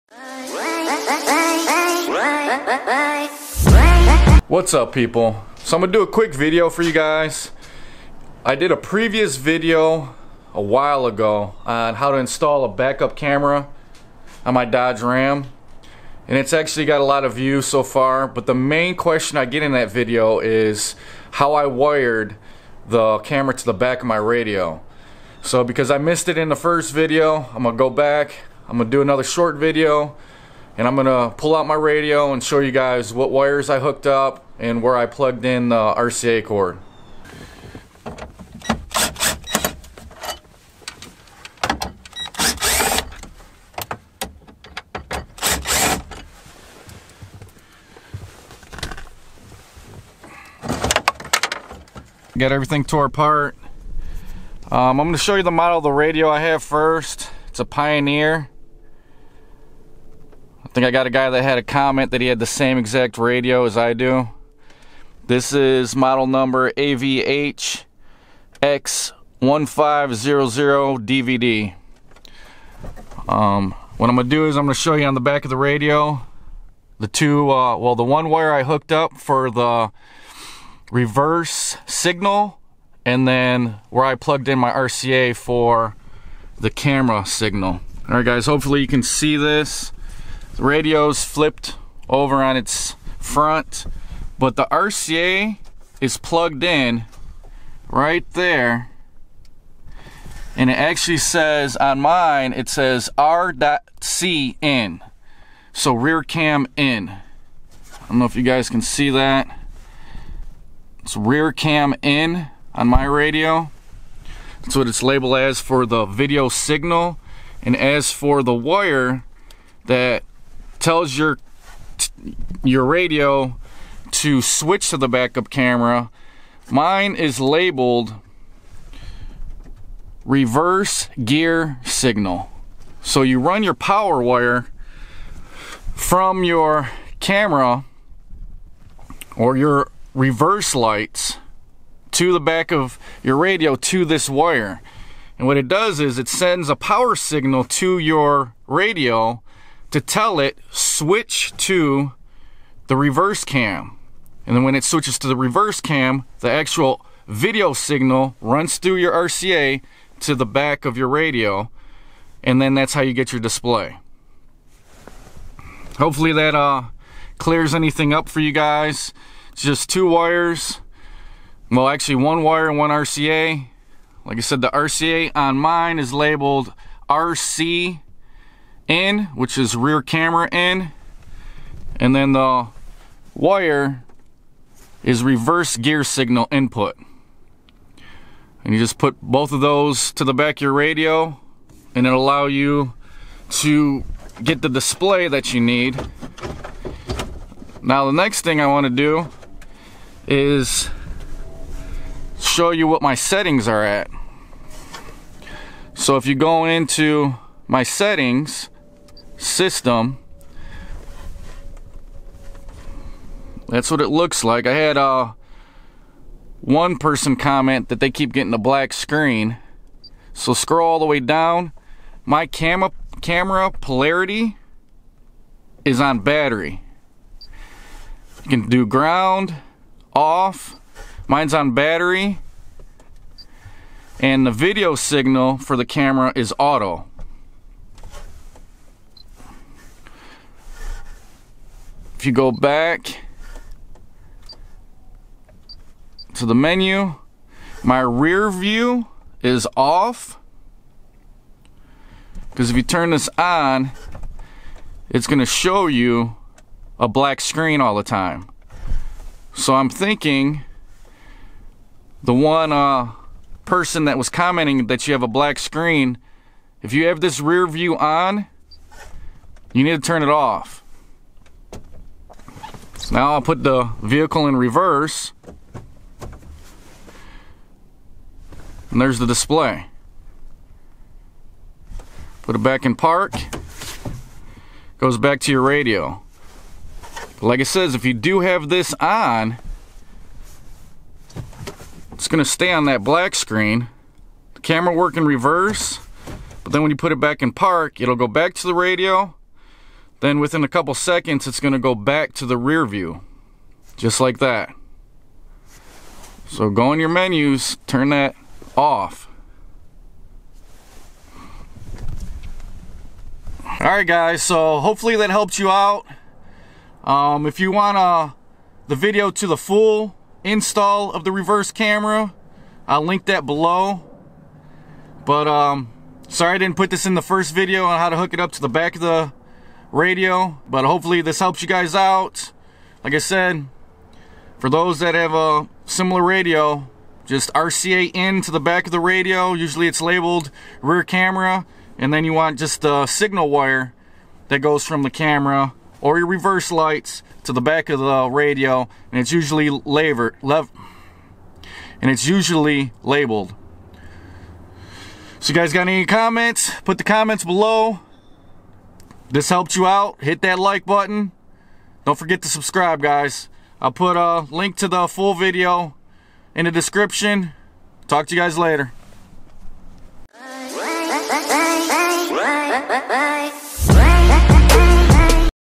How to wire a backup camera to your radio/indash screen and what settings to use
WHATS UP PEOPLE ! In this video i do a part two of a previous video i up loaded awhile ago on how to install a back up camera. After uploading that vid its got a lot of views and one main question is what about how you wired it to the back of the radio.
So i decided to go back over that in this video hope it answers some questions.

Also i figured i would give you guys an update on hw the camera is doing.. and its working as well as it did the first day no problems to report of !